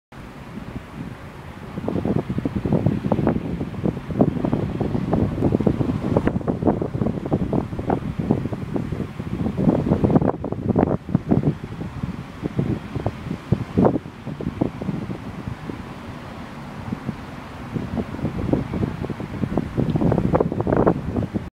iPod Touch 4g HD test
A test to see how the iPod Touch 4g is in low light, windy conditions.